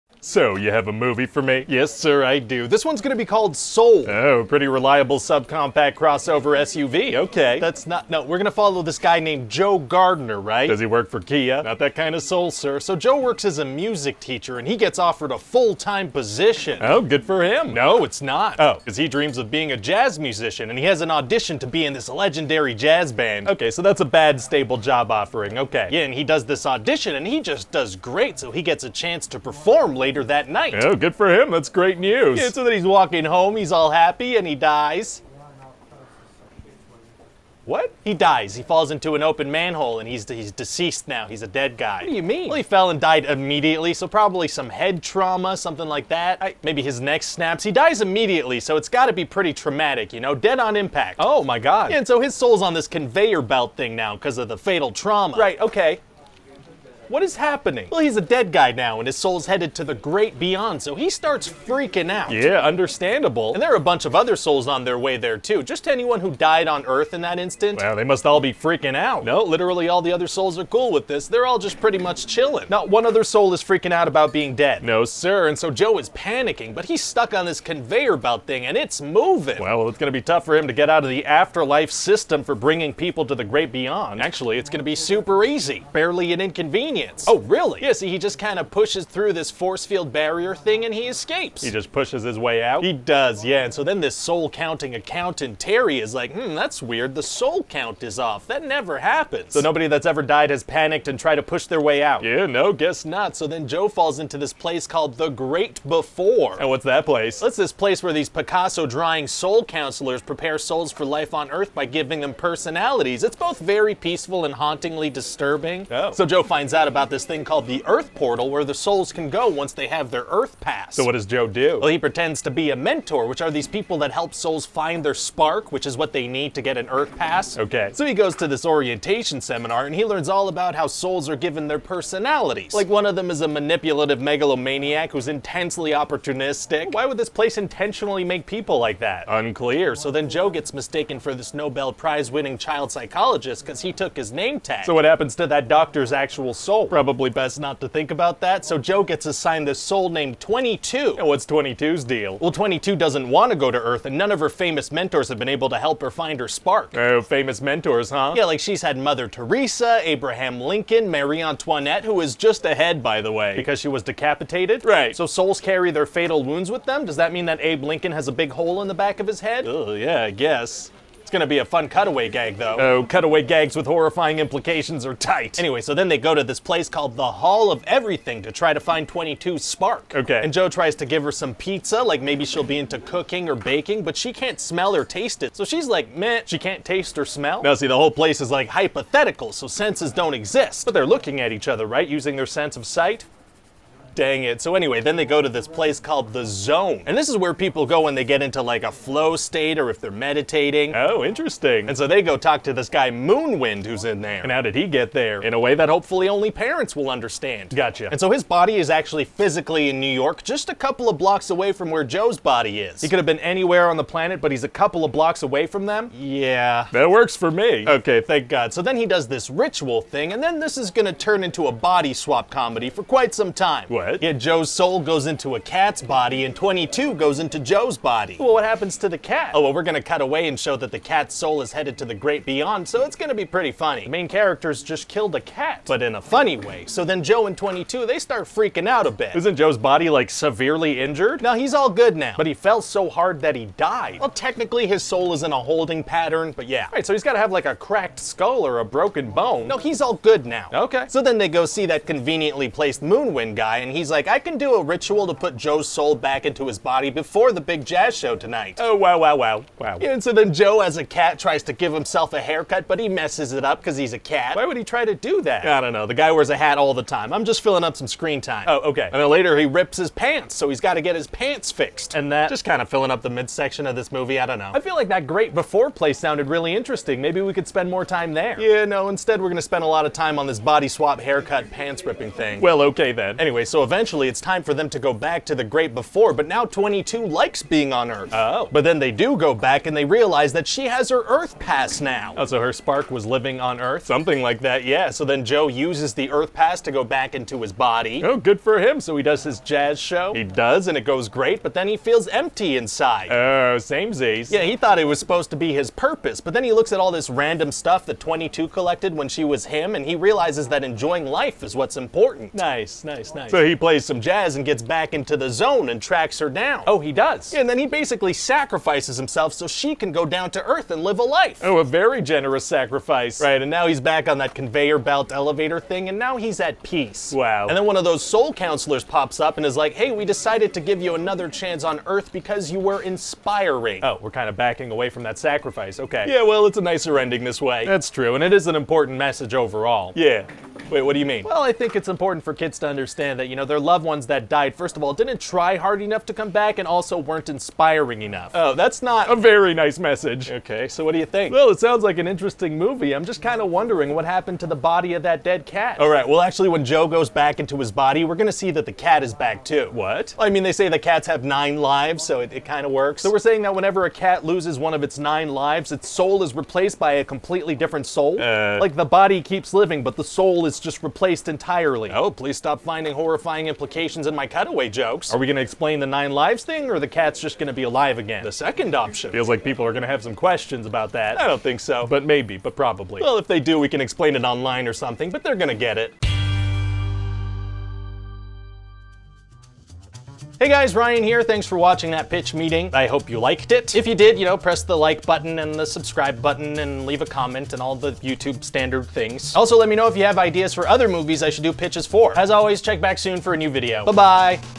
Soul Pitch Meeting
Step inside the pitch meeting that led to Pixar’s Soul!

The people at Pixar are master storytellers, and in recent years have been tackling some increasingly mature themes with movies like Inside Out and Coco. Their latest movie, Soul, starring Jamie Foxx and Tina Fey, tackles the subject of what happens to the soul after we die. These are still kid’s movies, right?

Soul does raise some questions though. Like what kind of injury did Joe sustain when he fell into the manhole? How is his body totally fine when he jumps back into it later? Why aren’t any of the other souls freaking out about their deaths? Is Joe really the first one to freak out and push on the wall a little? What happened to the soul of the doctor he impersonated? Or the cat for that matter? What kind of a message is that ending sending? Why is Marie Antoinette only a floating head?

To answer all these questions and more, step inside the Pitch Meeting that led to Pixar’s Soul! It’ll be super easy, barely an inconvenience.